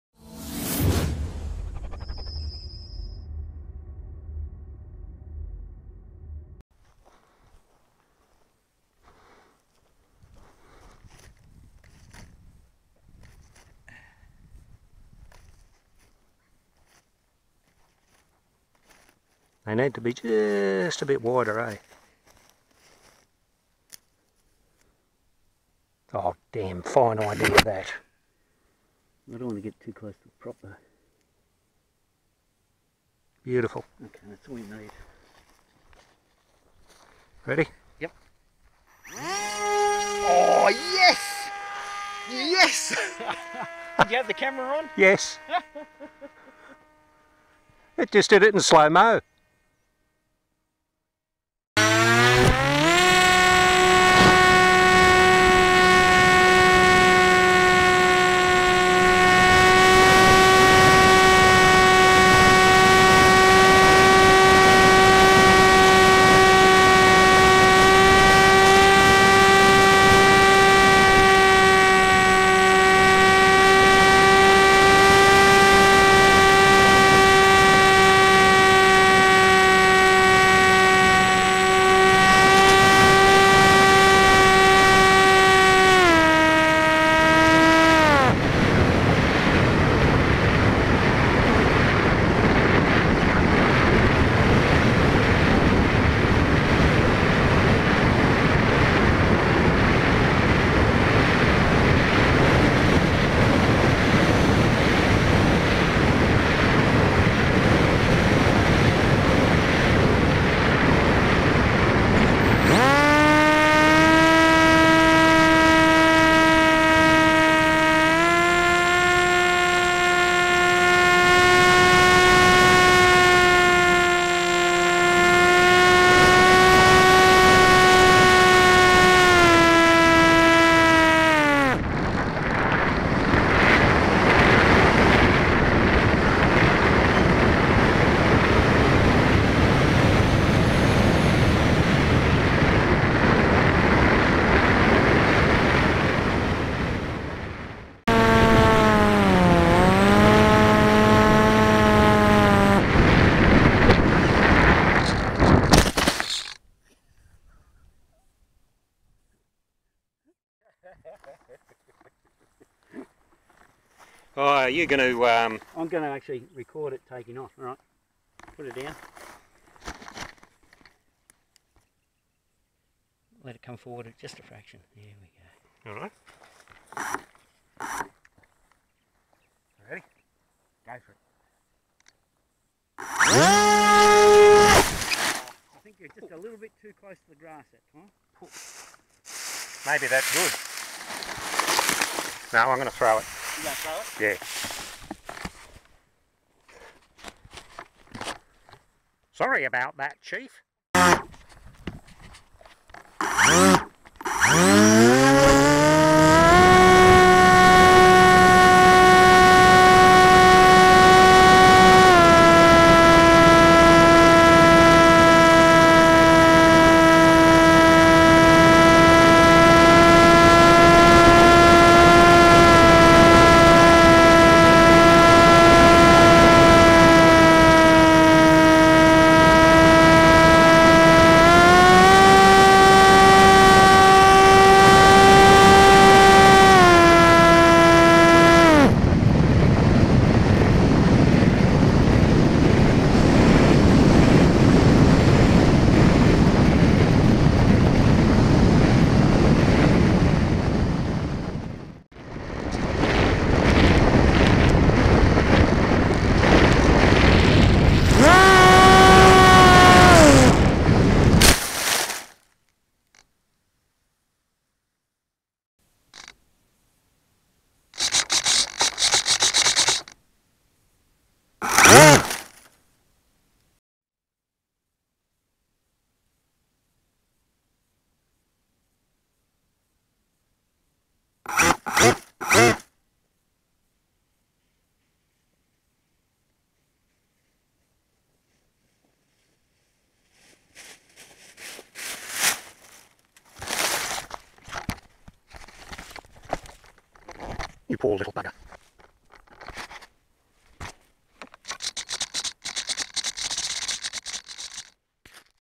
Mini Arrow and mobius
Some fun with the Mini Arrow, and the trusty old Mobius 1.
What a heap of fun we had. Like two kids with sky rockets !!